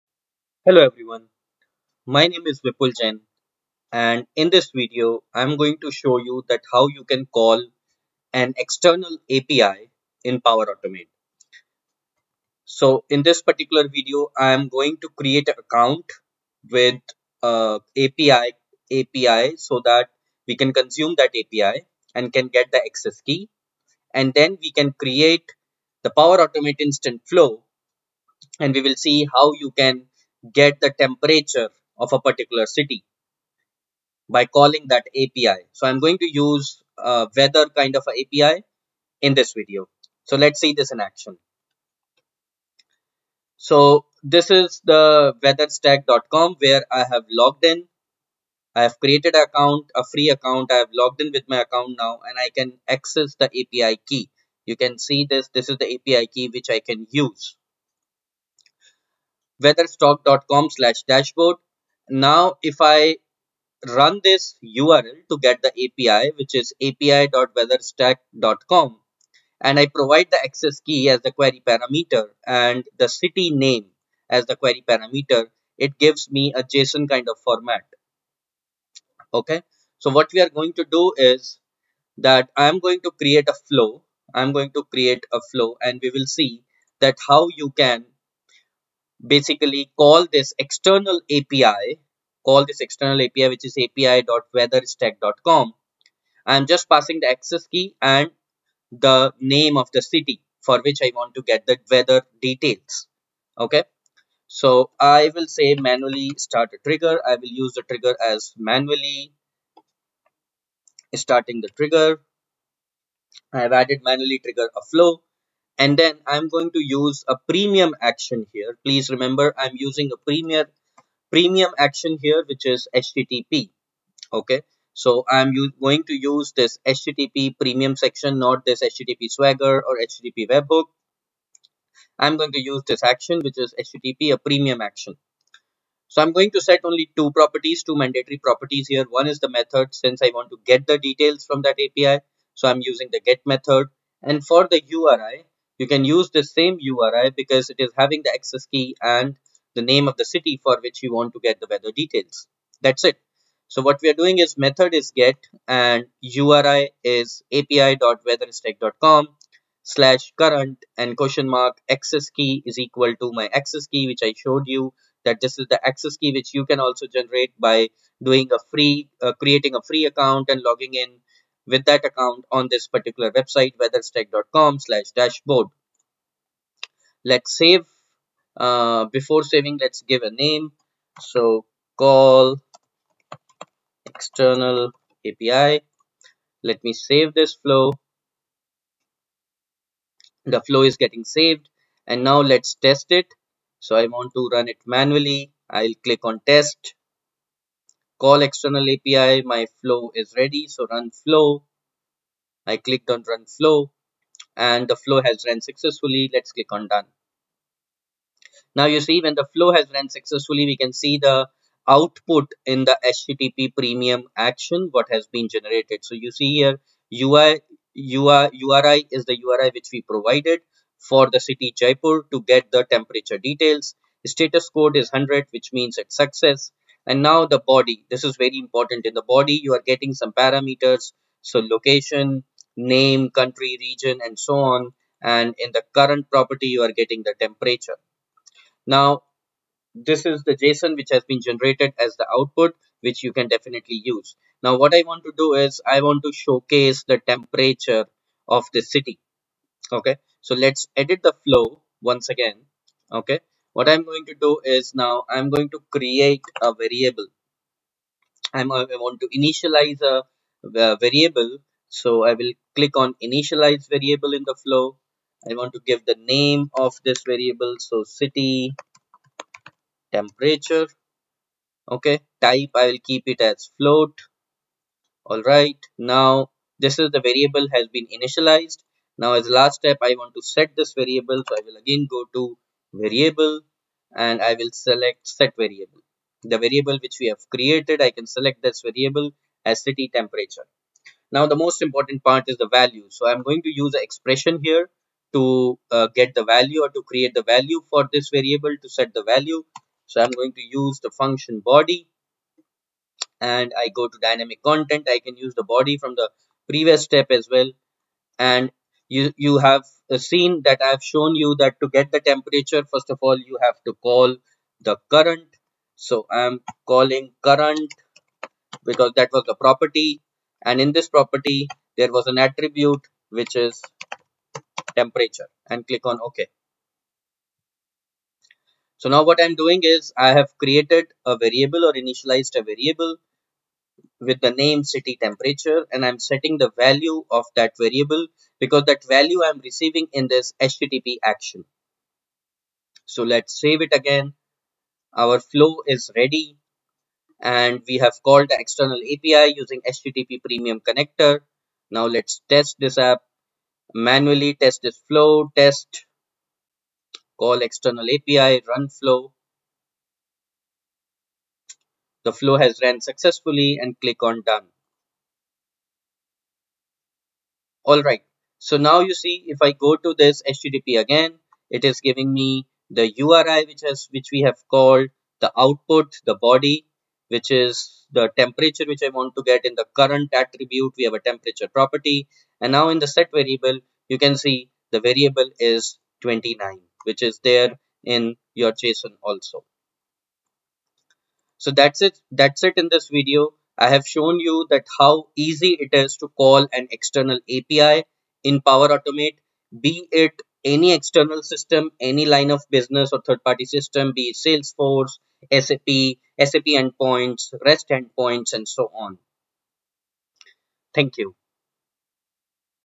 Call an external API using Power Automate
This video explains how to call Third Party (external) API using Power Automate HTTP Premium Action.